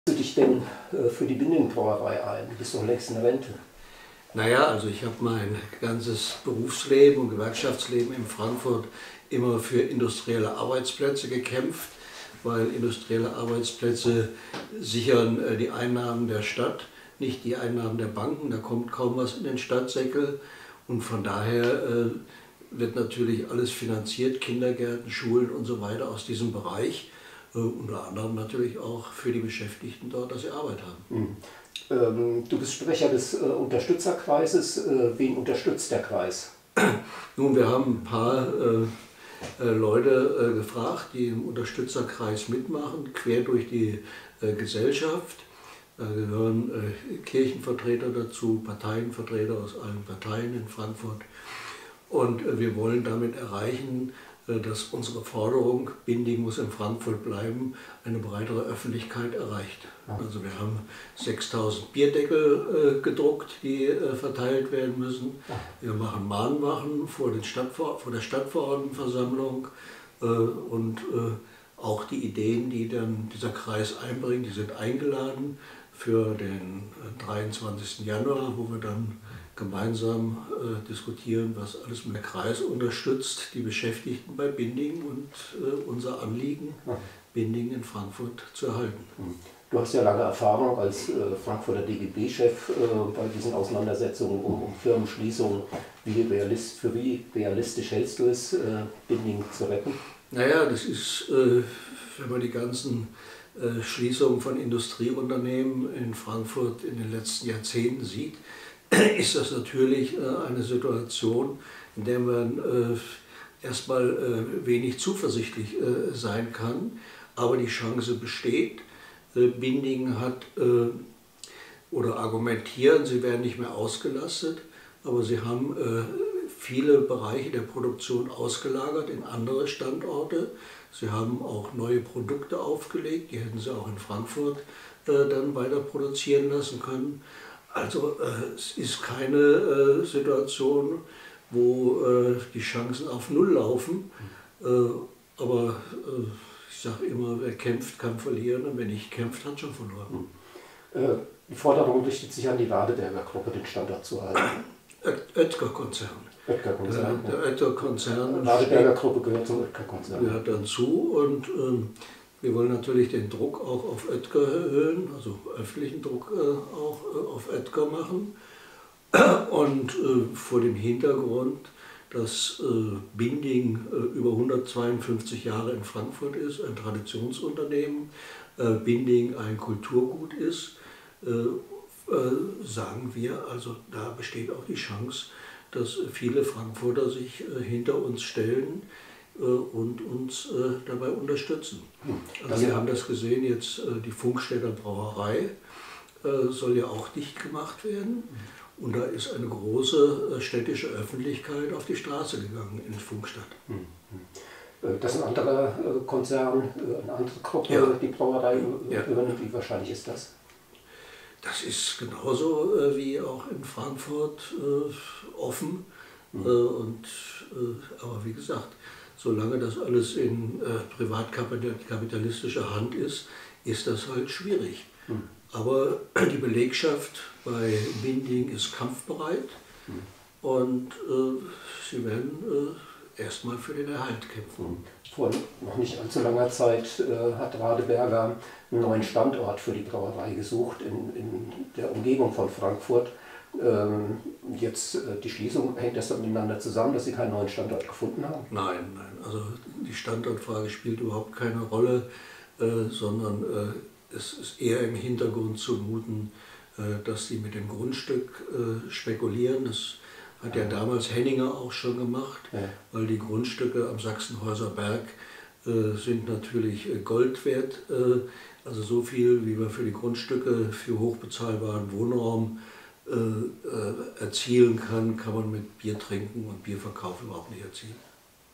Binding
Interview mit Harald Fiedler zur Schließung der Binding-Brauerei